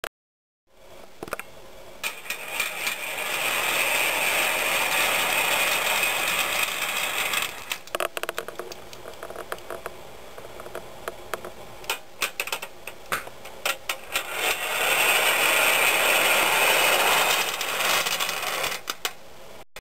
Copper Rolling Ball Sculpture 20091116
This is a rolling ball sculpture I made out of copper.  I sometimes sell sculptures like this on ebay or etsy.